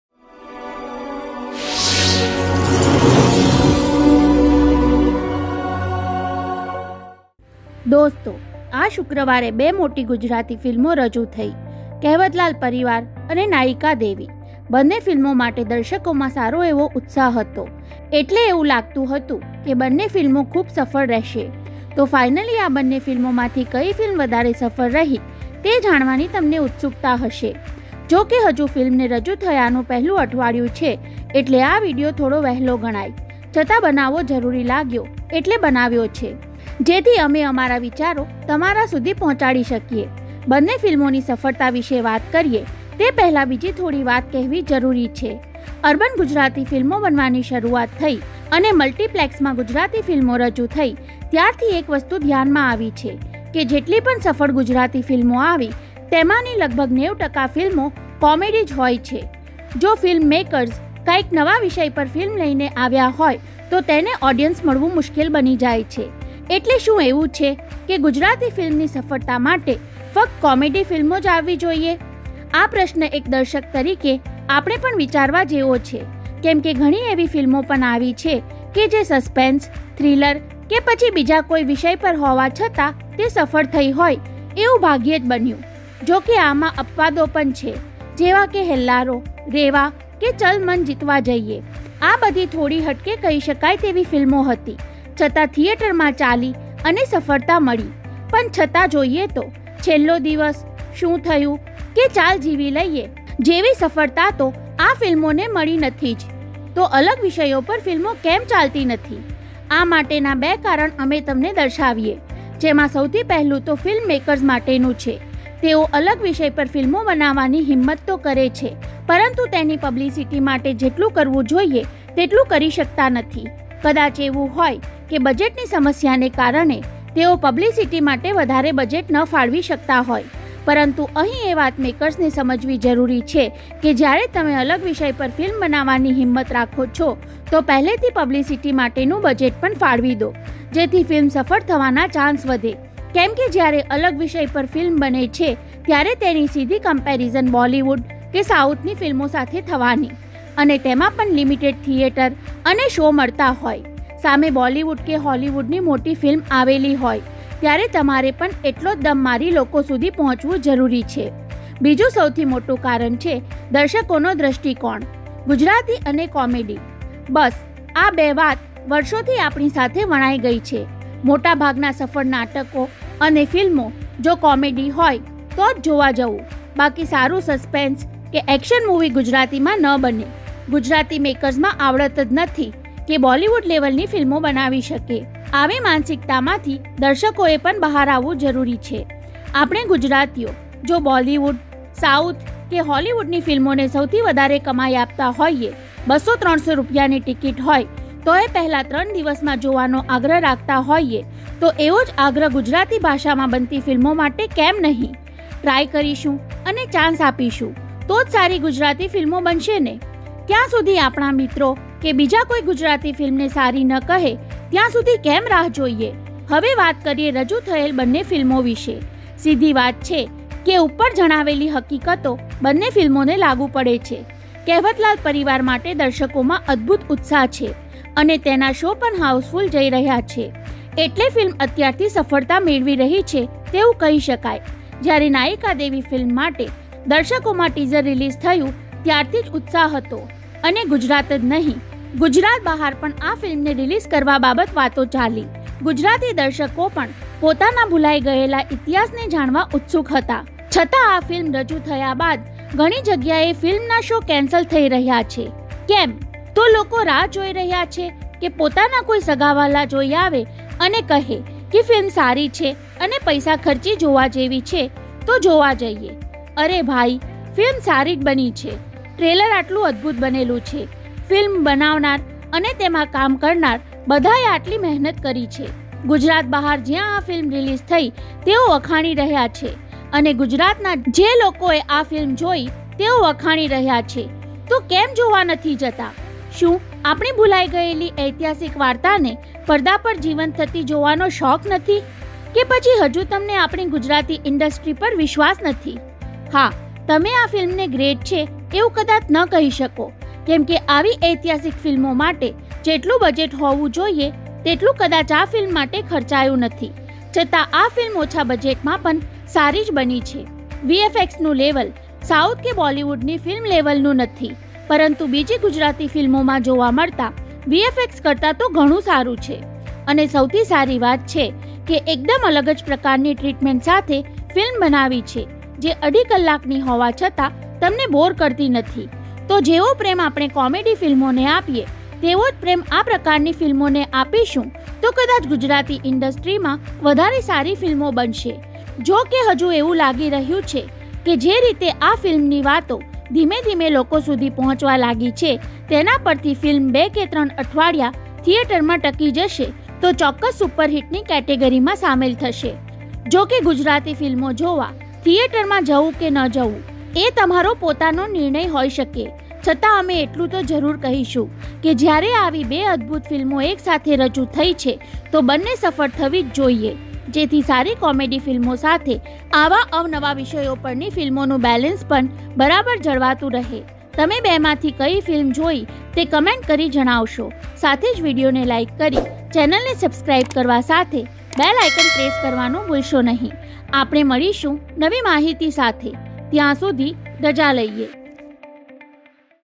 Kehvatlal Parivar V/s Nayikadevi: Who is the winner | Best Review | Nayika Devi
Kehvatlal Parivar vs Nayikadevi: Who is the winner | Best Review | Kehvatlal | Nayika Devi
'કેહવતલાલ પરિવાર' કે 'નાયીકાદેવી' કઈ ફિલ્મ વધારે સફળ?

Our Other Channel 'Audio Book Gujarati"